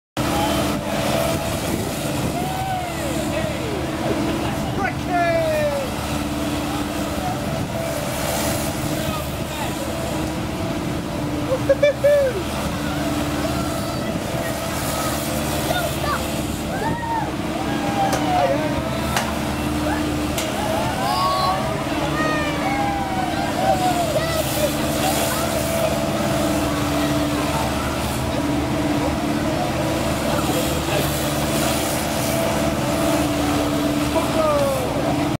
McJannett-Smith Kids and their Cousin Paul on Dig-A-Round in Diggerland 31st May 2010
This video shows another of the rides at Diggerland that my kids enjoyed with their cousin, it was called Dig-A-Round, just a digger converted in to a fairground ride but the kids loved it, James, Georgia and Paul all had a great time on it, whilst I was getting dizzy filming it all.